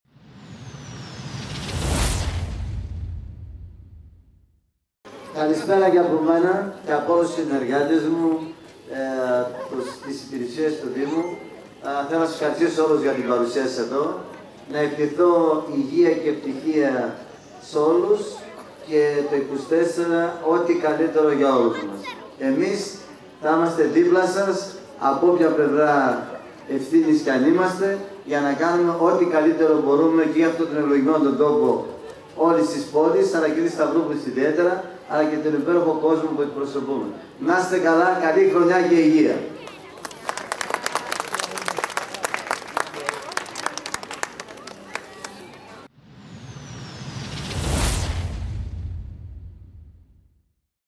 ΜΑΝΩΛΗΣ ΤΣΕΠΕΛΗΣ : "ΘΑ ΕΙΜΑΣΤΕ ΔΙΠΛΑ ΣΑΣ ΑΠΟ ΟΠΟΙΑ ΘΕΣΗ ΕΥΘΥΝΗΣ ΚΙ ΑΝ ΕΙΜΑΣΤΕ "
ΜΑΝΩΛΗΣ ΤΣΕΠΕΛΗΣ :  "...ΘΑ ΕΙΜΑΣΤΕ ΔΙΠΛΑ ΣΑΣ ΑΠΟ ΟΠΟΙΑ ΘΕΣΗ ΕΥΘΥΝΗΣ ΚΙ ΑΝ ΕΙΜΑΣΤΕ ΚΑΙ ΘΑ ΣΥΝΕΧΙΣΟΥΜΕ ΝΑ ΚΑΝΟΥΜΕ ΤΟ ΚΑΛΥΤΕΡΟ ΠΟΥ ΜΠΟΡΟΥΜΕ ΓΙΑ ΑΥΤΟΝ ΤΟΝ ΕΥΛΟΓΗΜΕΝΟ ΤΟΠΟ..."